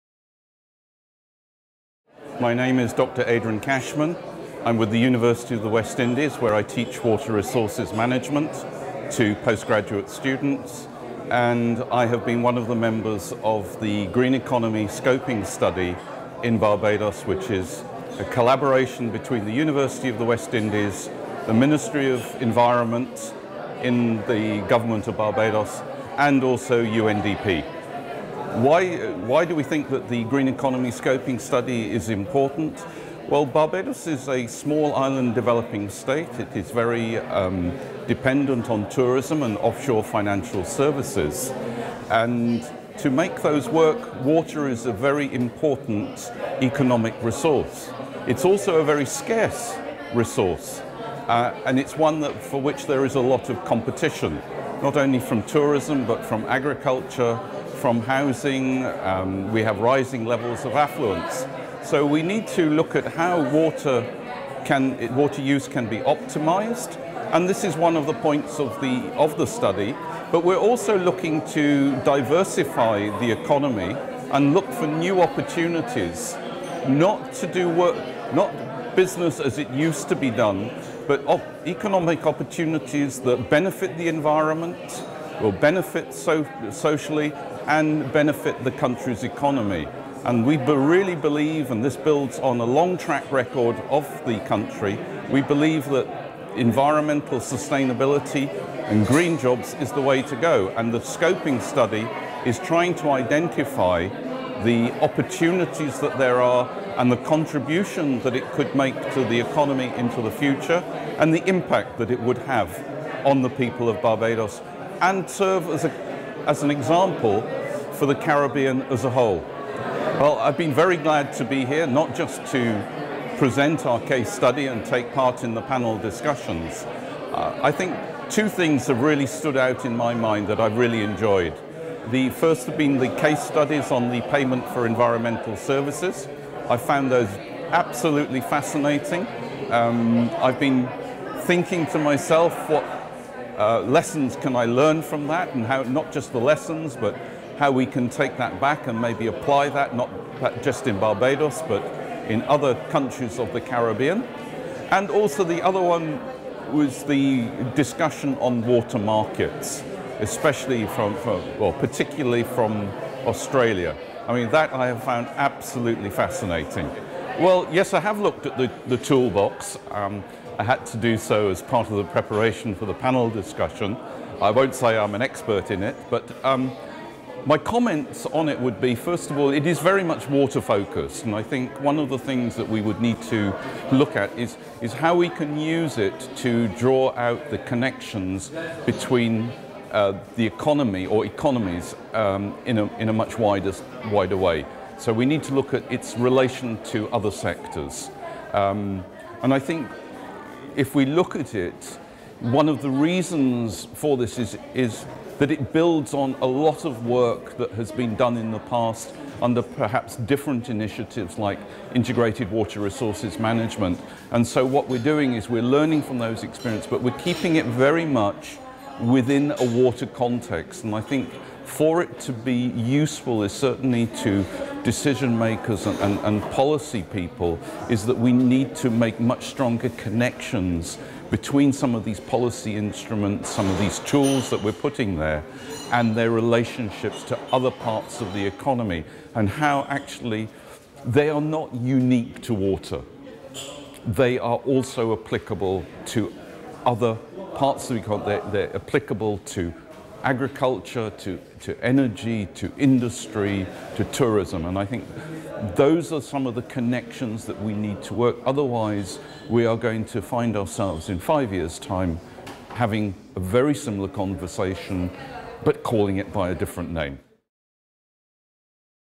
Adrian Cashman, Senior Lecturer in Water Resources Management. Barbados
Interviewing Adrian Cashman, Senior Lecturer in Water Resources Management, Centre for Resource Management and Environmental Studies (CERMES), University of West Indies, Barbados. Adrian presented a case on "The scope for greening Barbados' economy" at the International UN-Water Conference. Water in the Green Economy in Practice: Towards Rio+20 (Zaragoza, Spain, 3-5 October 2011).